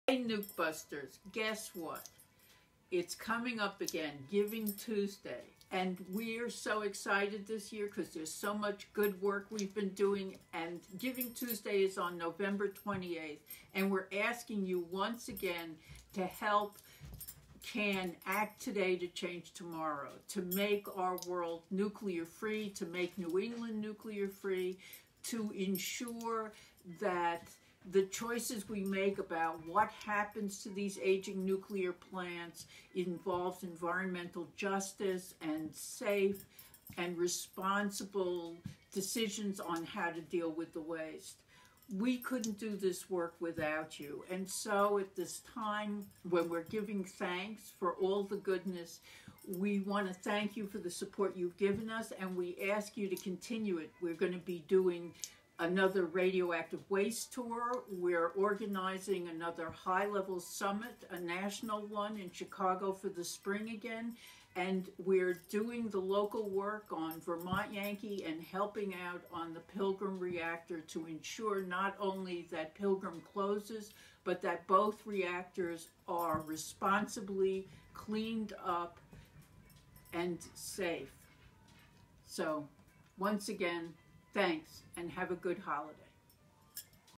Update from Deb Katz for Giving Tuesday 2017
We are at a tipping point in insuring that our children have a safe and clean future. Don’t let the Trump administration undermine all the good work that we have done.
GivingTuesday on November 28, 2017! Help CAN working on a nuclear free future